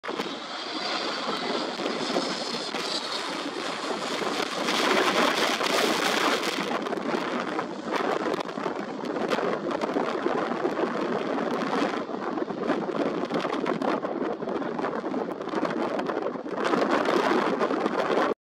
Hard Landing & Go Around, Engine Compressor Stall ! At Funchal, Madeira
No flight review this time, just a short clip about a recent incident at FNC.

A full Madeira Airport spotting experience I will post later, in a full length video, with other go-arounds due to winds and many successful landings of course.